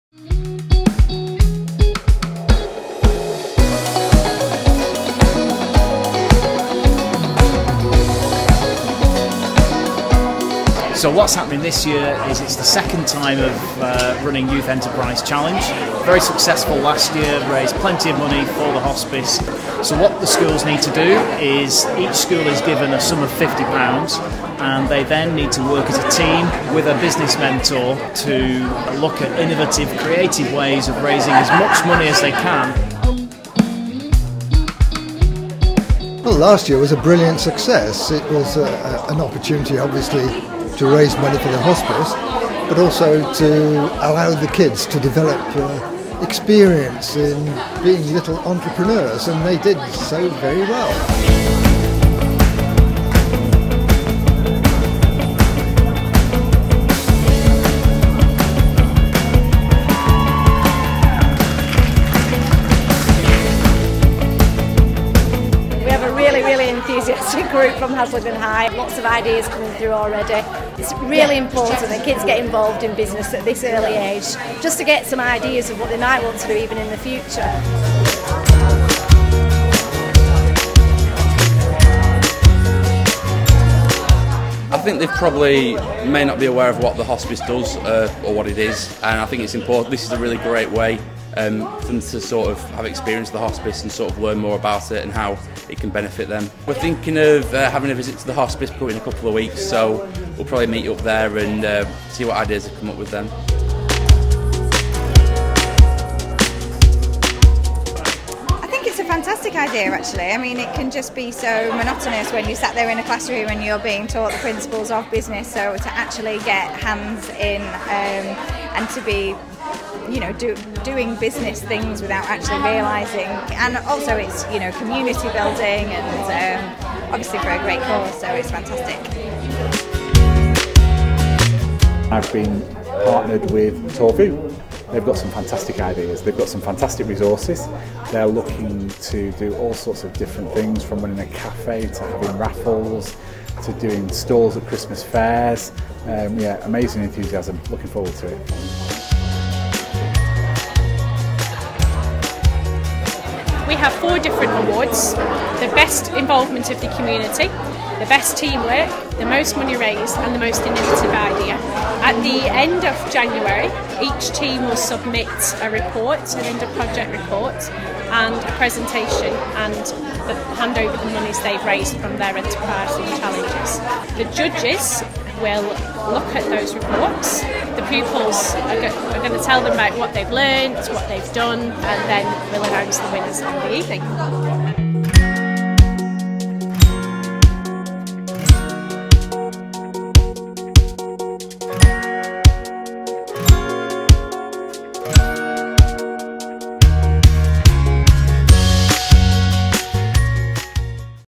Rossendale Hospice Youth Enterprise Challenge 2016 - Launch Event
The Youth Enterprise Challenge will see teams from six schools including Alder Grange, All Saints, Fearns, Haslingden High, Tor View and Whitworth High come up with enterprising initiatives, with all the proceeds going to Rossendale Hospice.

During the three-month challenge, the Year 10 school teams will link up with a mentor from a local business, who have kick-started their project with a £50 donation.